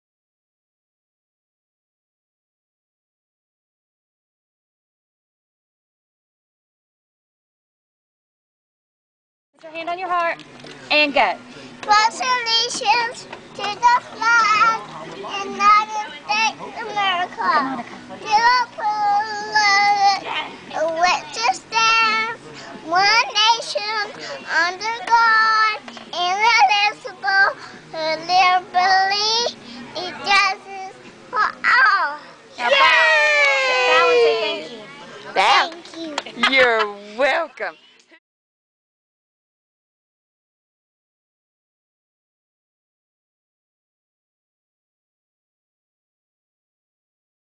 Angel Carter, age 2, recites The Pledge of Allegiance
Simply Adorable...